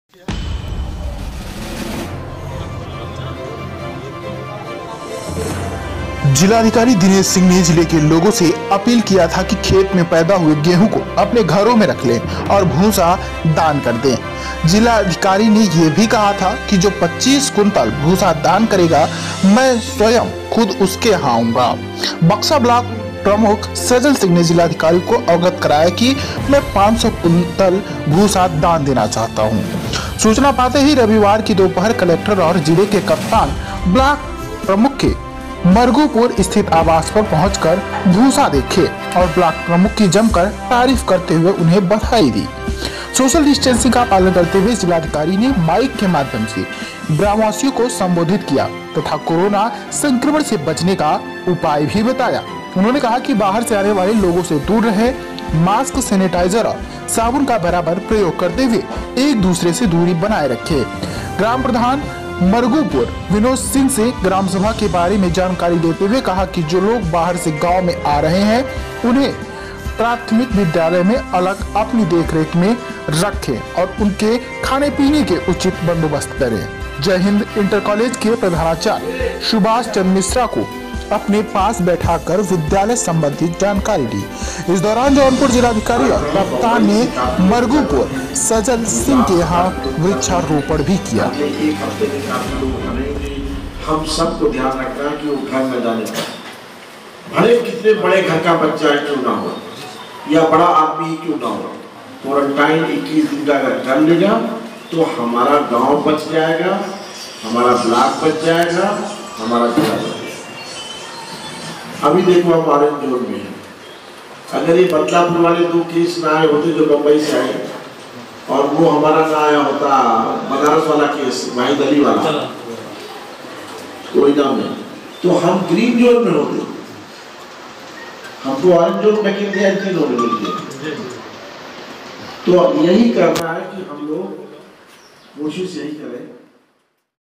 बक्शा/जौनपुर:भूसा बैंक में ब्लॉक प्रमुख सजल सिंह द्वारा पाँच सौ कुन्तल भूसा किया गया दान
रिपोर्ट:-दिलशाद अहमद